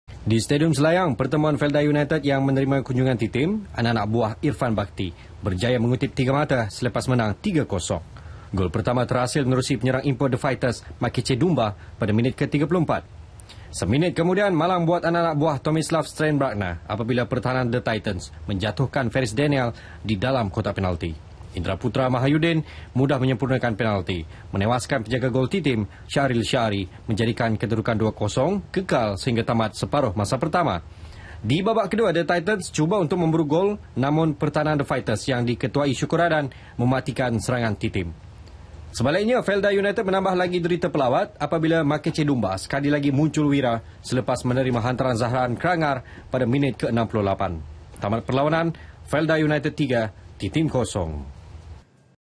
FELDA TEWASKAN T.TEAM 3-0 [13 SEPT 2015]
Di Stadium Selayang, pertemuan Felda United yang menerima kunjungan T.Team, anak-anak buah Irfan Bakti berjaya mengutip 3 mata selepas menang 3-0.

Gol pertama terhasil menerusi penyerang import The Fighters, Makeche Ndumba pada minit ke-34.

Seminit kemudian, malang buat anak-anak buah Tomislav Steinbruckner apabila pertahanan The titans menjatuhkan Ferris Denial di dalam kotak penalti.